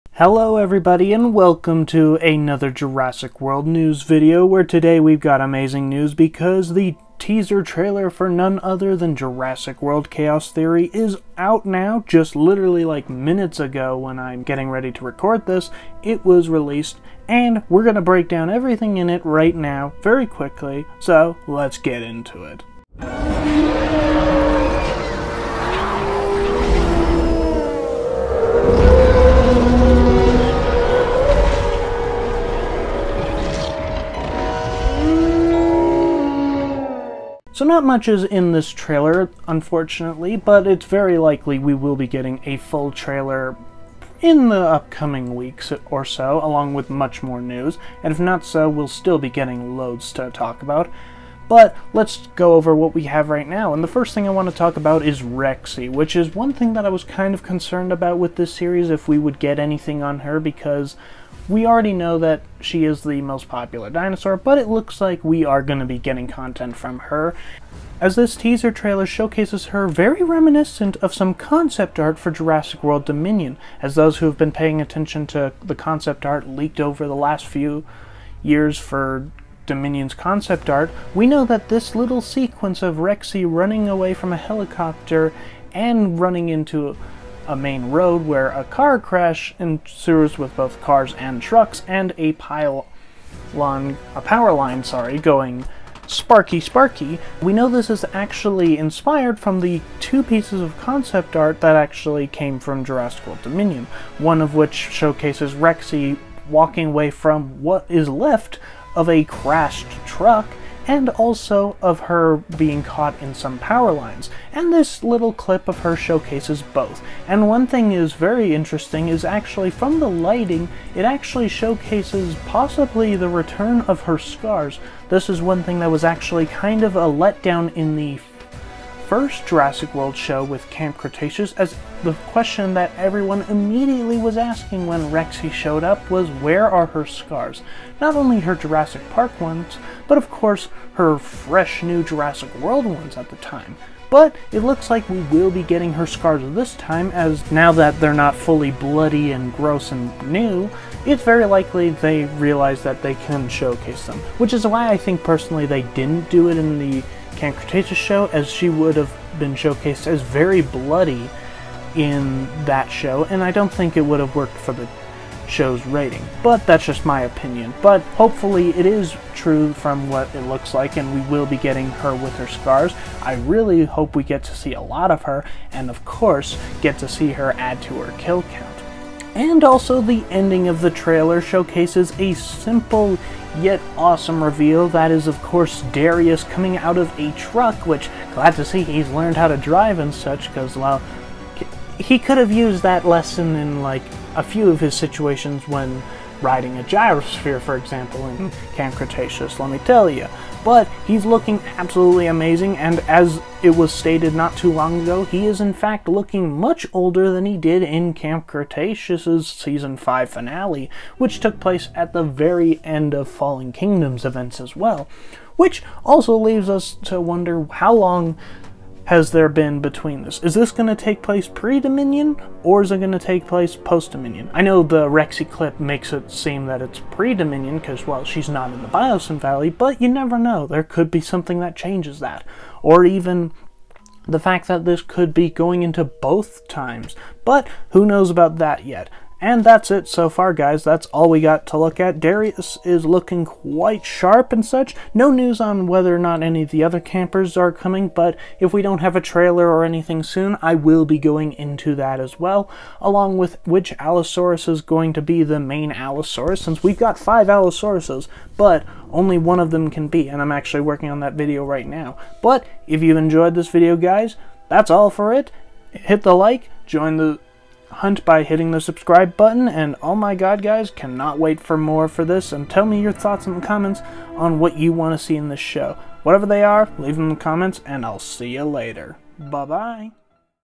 FULL TRAILER BREAKDOWN! | JURASSIC WORLD CHAOS THEORY TEASER TRAILER ANALYSIS
The first Teaser Trailer for Jurassic World: Chaos Theory has arrived and it look’s glorious from Rexy to  Darius to another dinosaur being possibly confirmed for the show and we’re going to breakdown everything in the trailer.

Help out the channel by